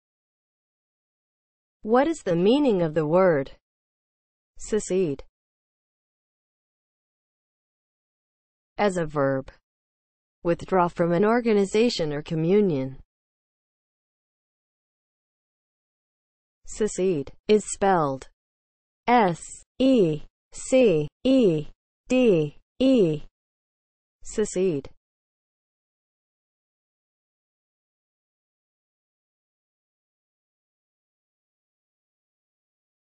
What is the meaning of the word SECEDE?
Definition and spelling of the word SECEDE.

(00:00) Intro
(00:07) As a verb
(00:14) Spelling

As a verb (00:07):
• withdraw from an organization or communion